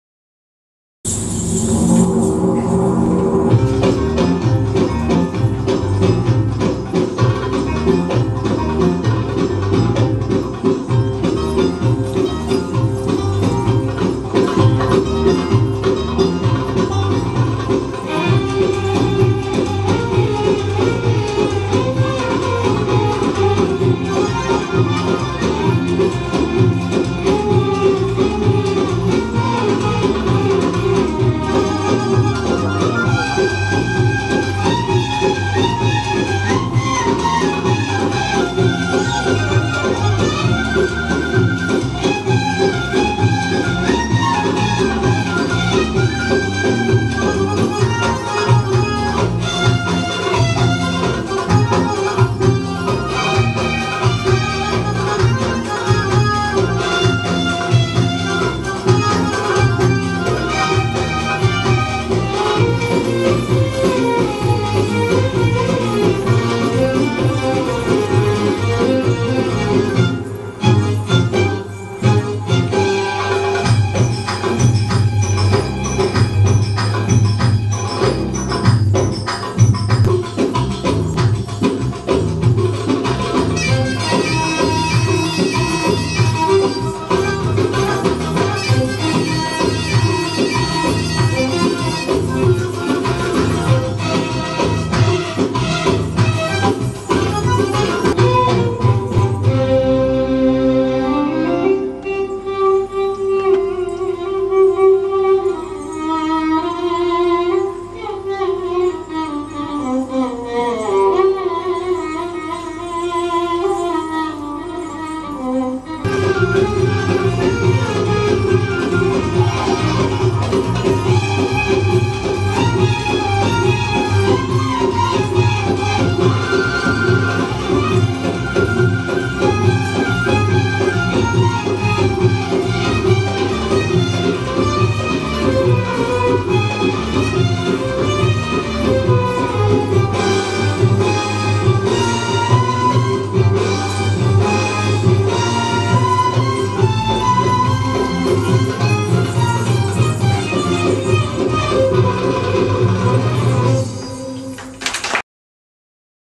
Variációk Dupla fátyollal Nurgül 2008
A következő előadás az NSDT Keleti Táncszínház az Elemek Harca című műsorszámából egy részlet!A piros és a kék fátyol jeleníti meg a tűzet és a vízet melyek állandó harcban állnak egymással és ezáltal örzik az állandó egyensúlyt, harmóniát is. A koreográfiát Nurgül Seymen készítette.